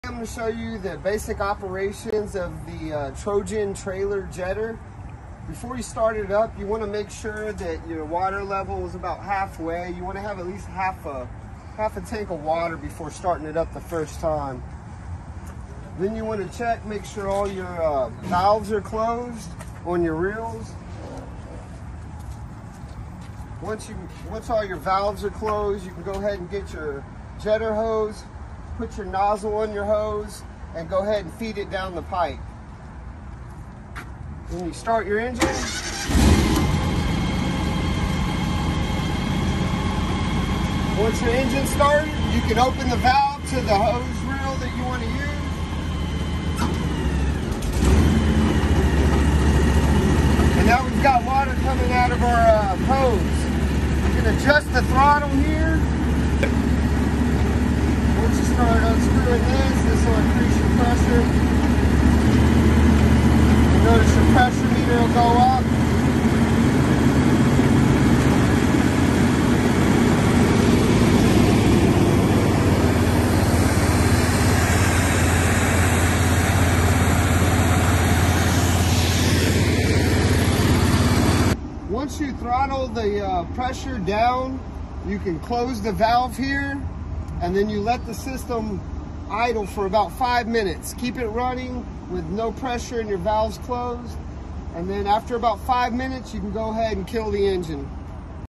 Operation instructions for the Trojan TW2000 Trailer Jetter
1. Fill tank at least half way full
2. Make sure all valves are in the closed position
3. Put nozzle on hose and then put hose in pipe
4. Start Engine
5. Open valve to the reel you are using
6. Use throttle to adjust pressure
7. When you reach the clog pull hose back & forth and let the hose do the work
8. When finished close the valve
9. Use automatic reel to bring the hose back
10. Let engine run idle for 5 minutes before turning off